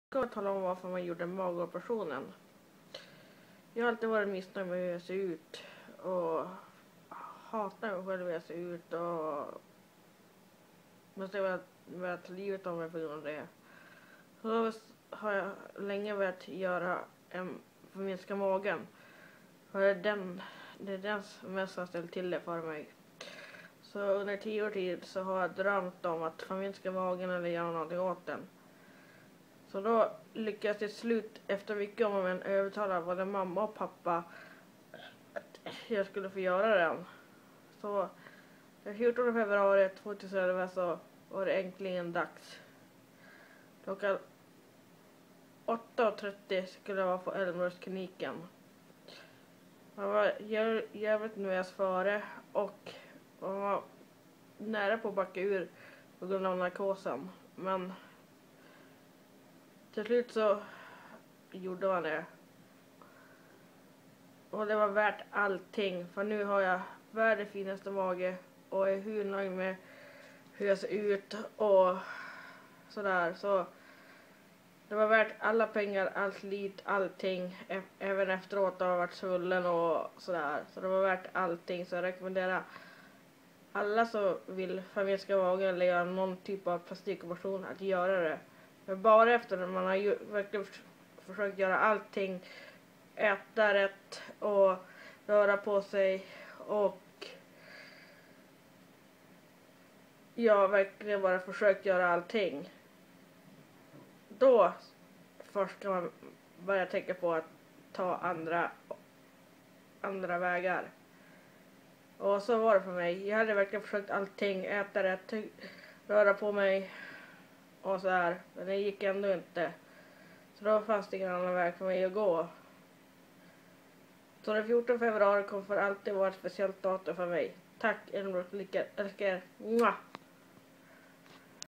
Magoperationen!
Jag berättar varför man gjorde den :)